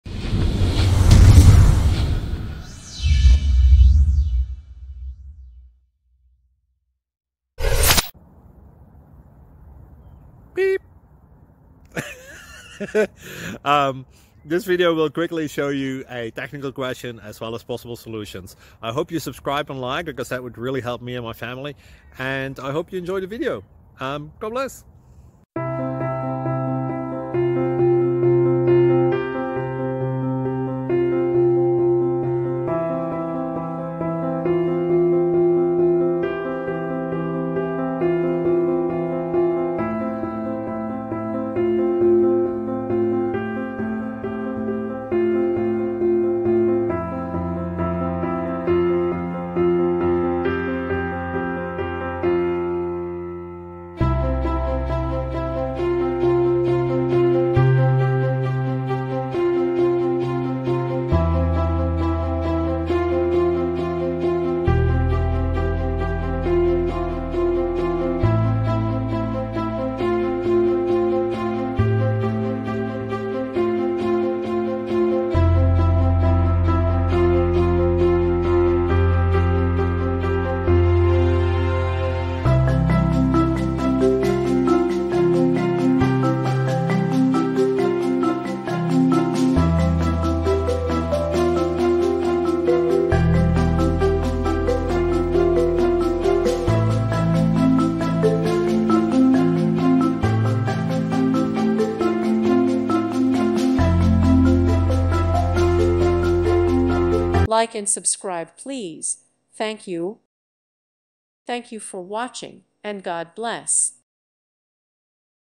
GIS: How to find the minimum pixel value within a region of interest? (2 Solutions!!)
GIS: How to find the minimum pixel value within a region of interest?

With thanks & praise to God, and with thanks to the many people who have made this project possible! You are responsible for your own actions.